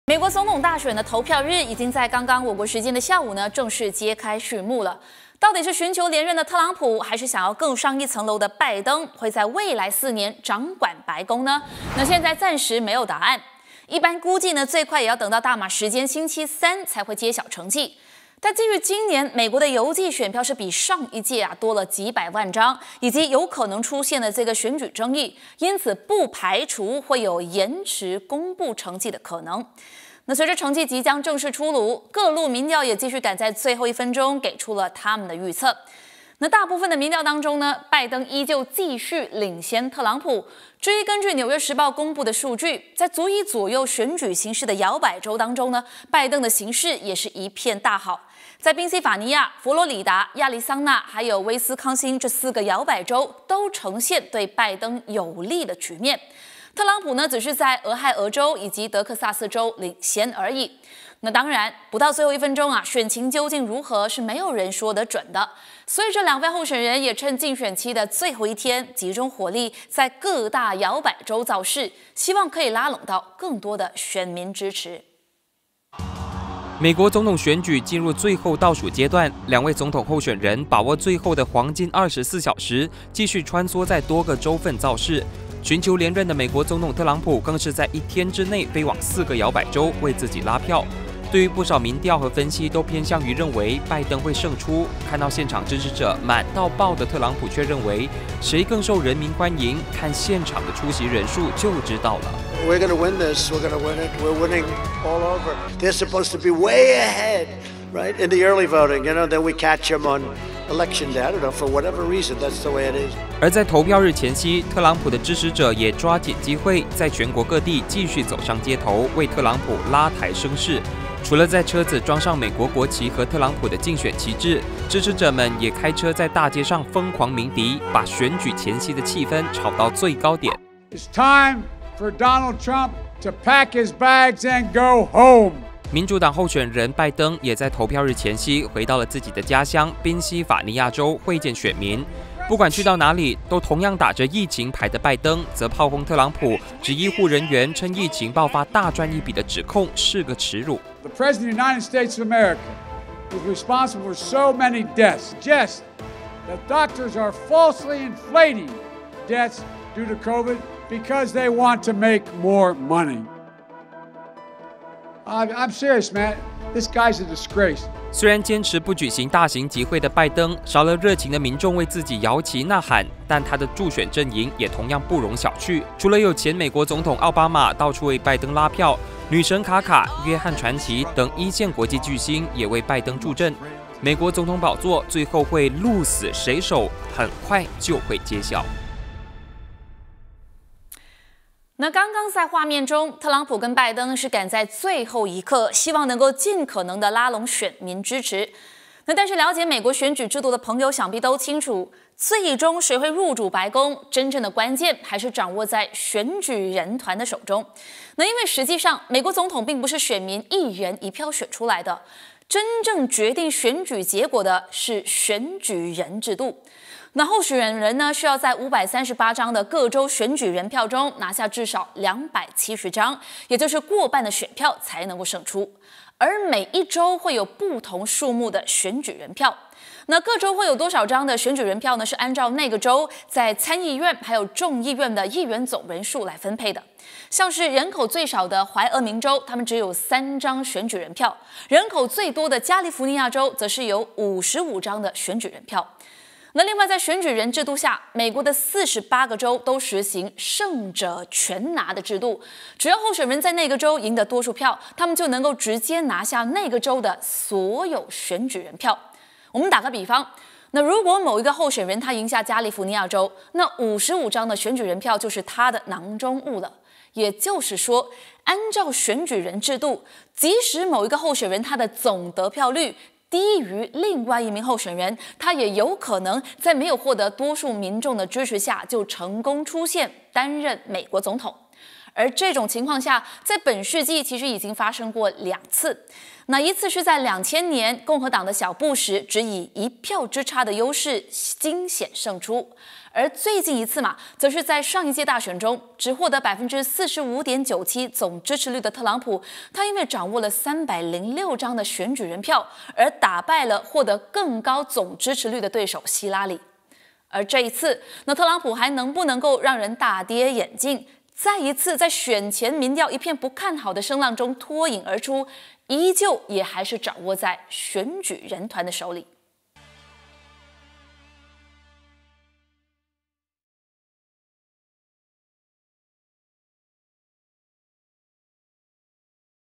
八点最热报 03/11/2020 6摇摆州拜登领先4个 特朗普能迎头赶上？
随着成绩即将正式出炉，各路民调也继续赶在最后一分钟，给出了他们的预测。大部分的民调中，拜登依旧继续领先特朗普。至于根据《纽约时报》公布的数据，在足以左右选举形势的摇摆州当中，拜登的形势也是一片大好。在宾夕法尼亚、佛罗里达、亚利桑那和威斯康星这四个摇摆州，都呈现对拜登有利的局面，特朗普只是在俄亥俄州以及德克萨斯州领先。当然，不到最后一分钟，选情究竟如何，没有人说得准。